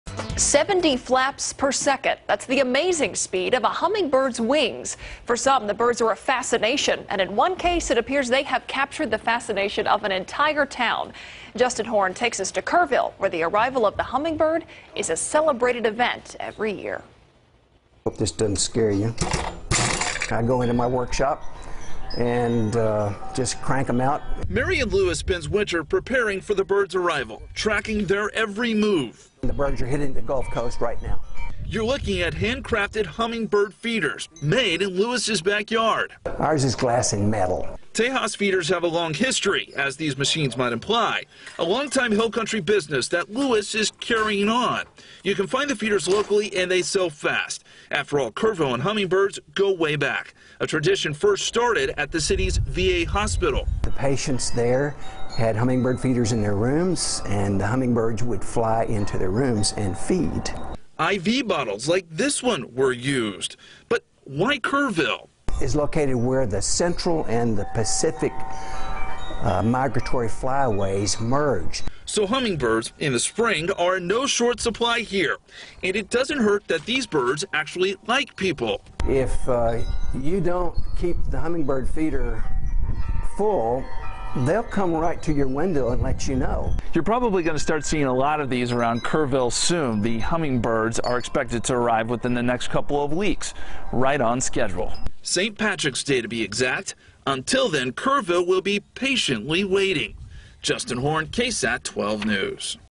Video: Kerrville man prepares for annual arrival of hummingbirds
With the annual arrival of migrating hummingbirds expected to come soon, Kerrville resident Marion Lewis is ramping up his bird feeder business.